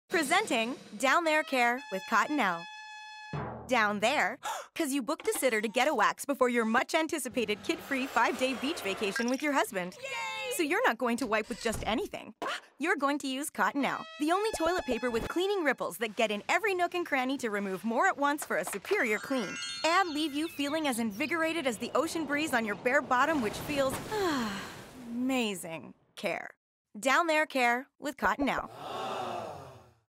Cottonelle Launches downtherecare
People don’t mind conversation around “down there” care; in fact, they embrace it. In a new survey from Cottonelle® brand, nearly half of Americans reported having a daily down there care routine.1 To further the conversation, Cottonelle® brand is launching the downtherecare program to urge all Americans to treat the skin they don’t see as well as the skin they do see.
The brand is confident its CleaningRipples™ Texture, featured across both Cottonelle® toilet paper and flushable wipe products, could be the catalyst to transform unconscious bathroom routines to mindful downtherecare. The texture removes more at once for a superior clean* and is backed by nearly four years of product innovation and development.